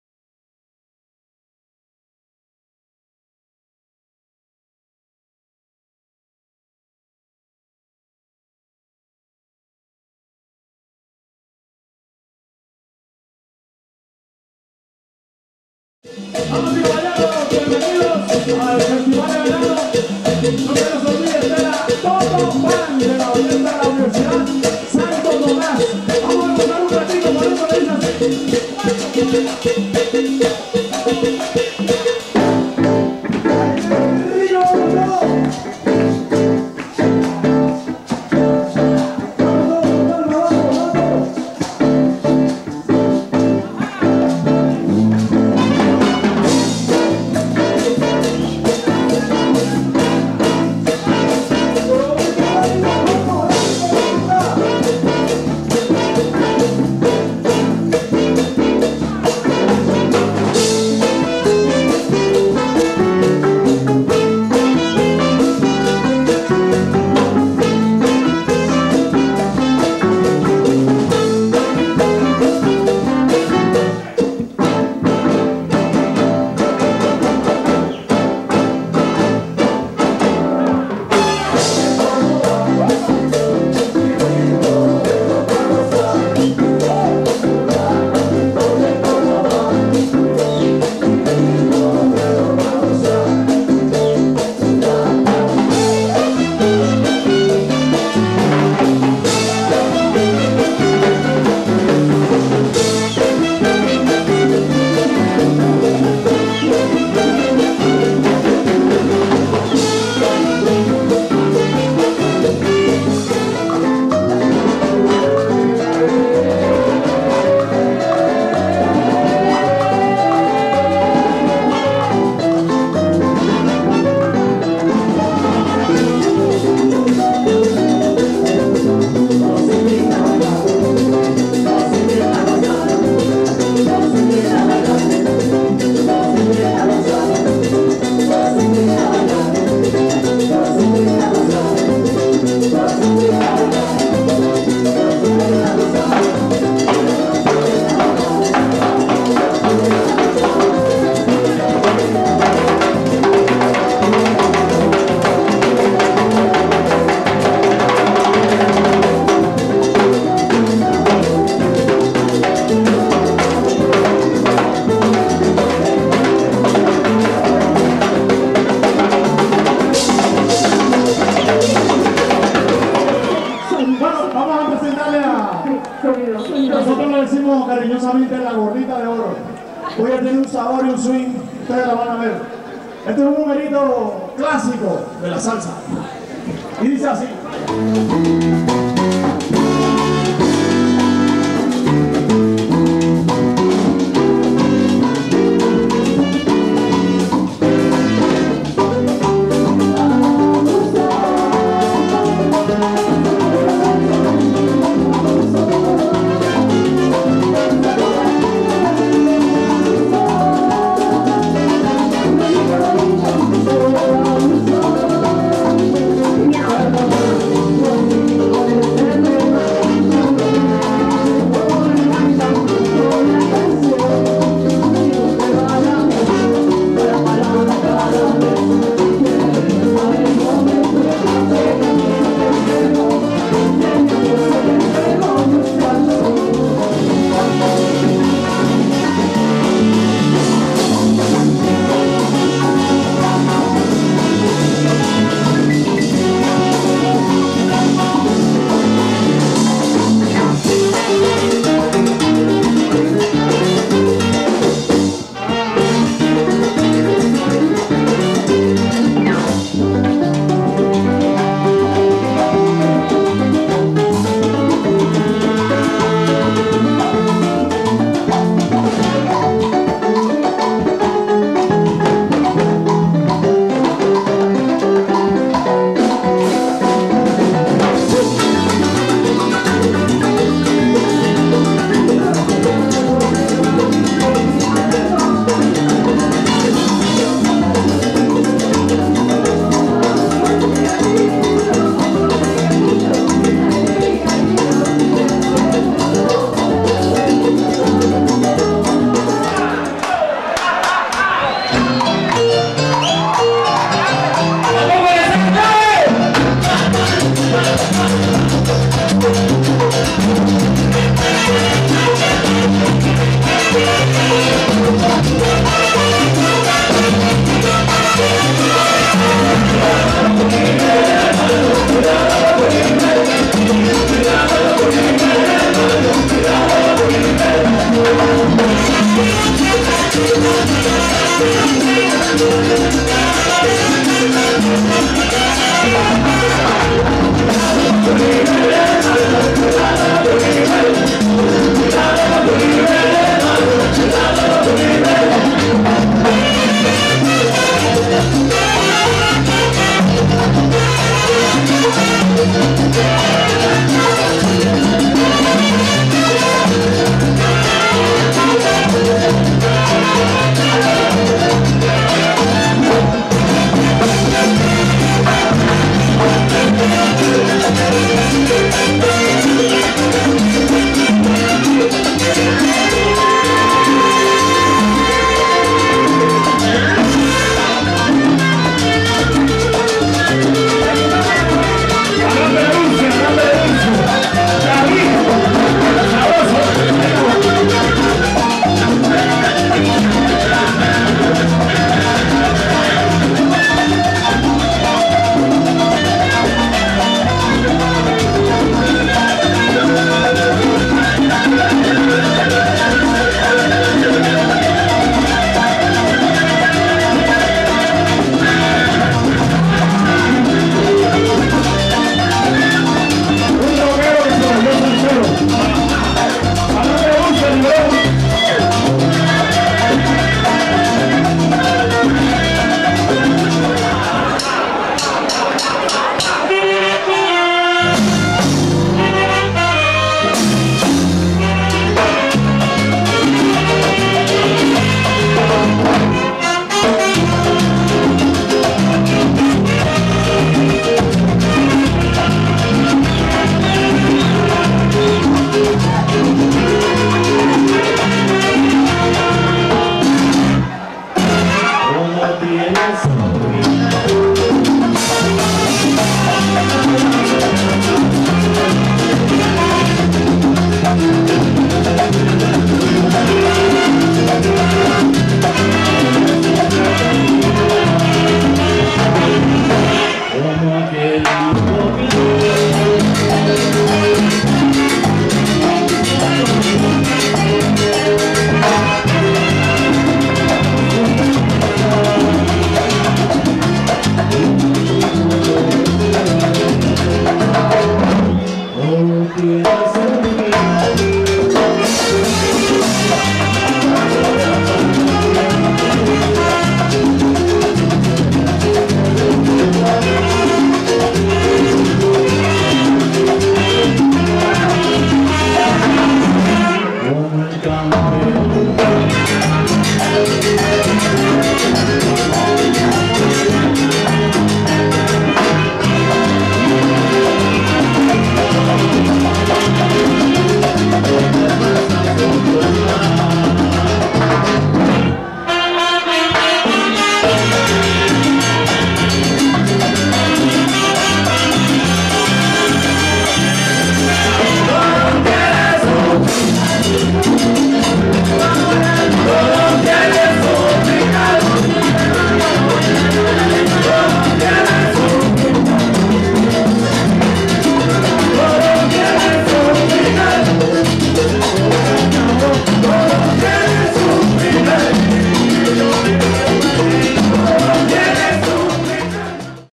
Orquesta La TotoBand: III Parte
Imágenes de la Orquesta universitaria La TotoBand perteneciente al departamento de Bienestar Universitario de la Universidad Santo Tomas. Recopilación de algunas presentaciones